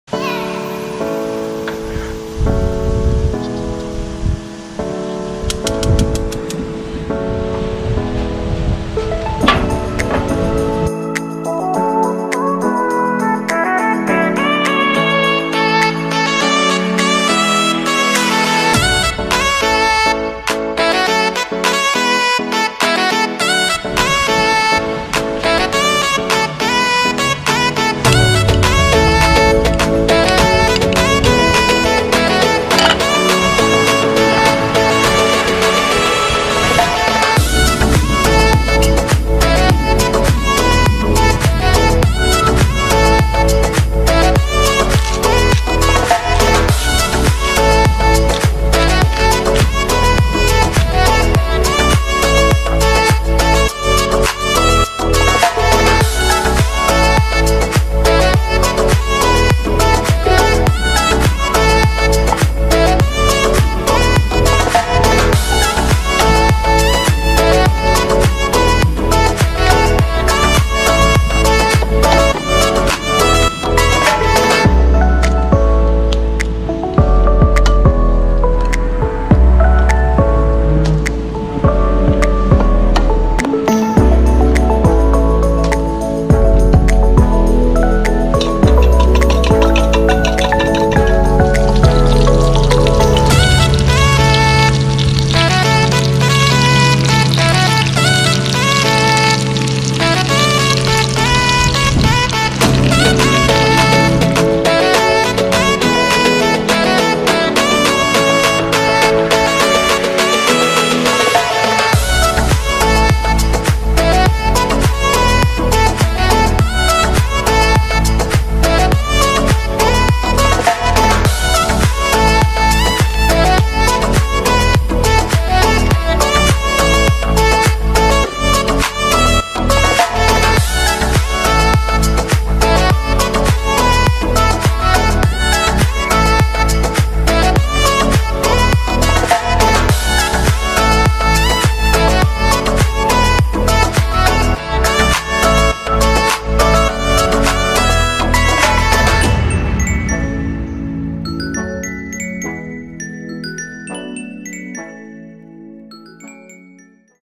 STEAK AND EGGS BREAKFAST PLATE * ROBERT ROY
The ultimate steak and eggs breakfast plate with some onions and mushrooms on a flat top griddle. Blackstone Griddle breakfast recipe.